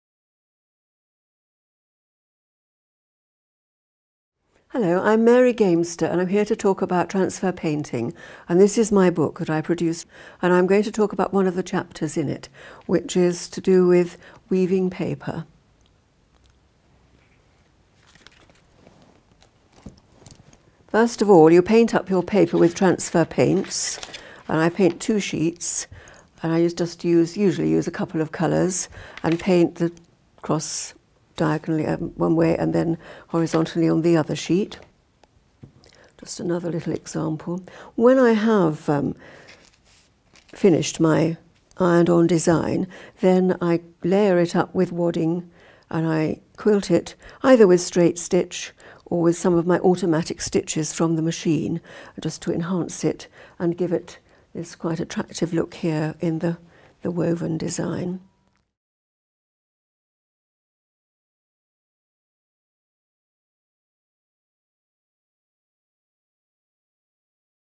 Paper Weaving and Transfer Painting with Mary Gamester (Taster Video)
Paper Weaving and Transfer Painting with Mary Gamester - in this workshop, Mary shows us how to pre-paint papers with transfer paints.  You’ll discover just how exciting and unpredictable the end result is.